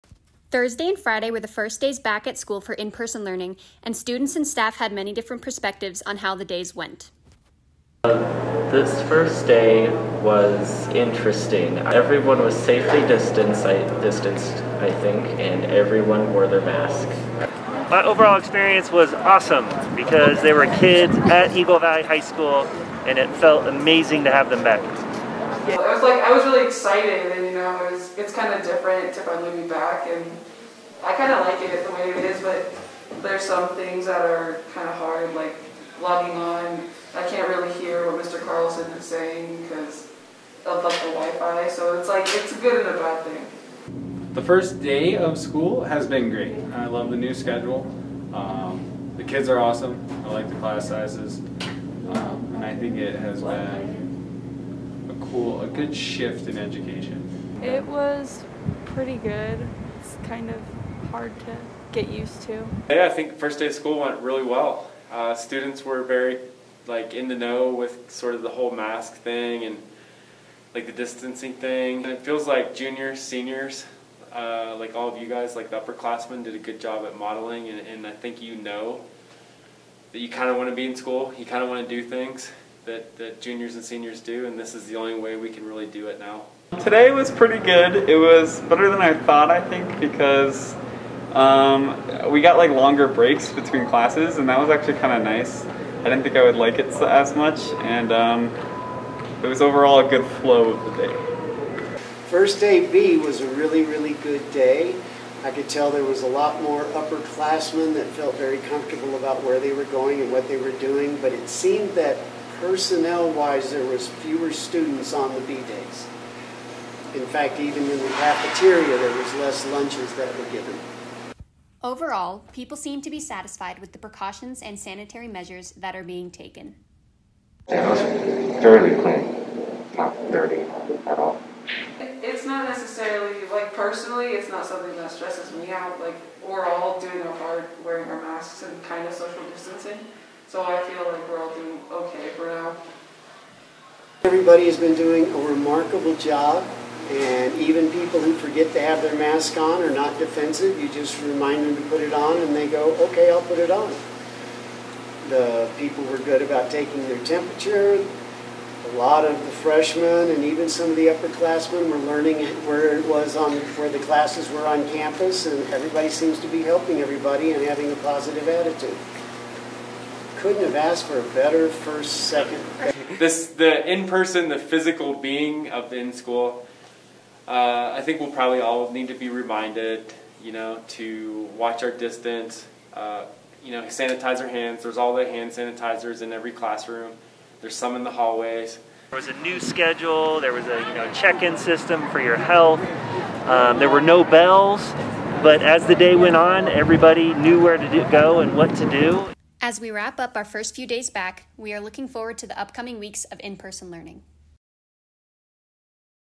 First Days of In Person Learning 2020-21
EVTV is back for the 2020-21 school year with reporting on Eagle Valley High School's first days of in person learning yesterday and today.

Reporting by Otto Berga, Sam Elliott, Langston James, Adriana Gallegos Helguera, Zoe Greener, Sam Heller, Arden Houck, Alex LeBarron, Jordan Neifert, Kimberly Mayorga Escalante, and Zosia Skiba
Editing by Kimberly Mayorga Escalante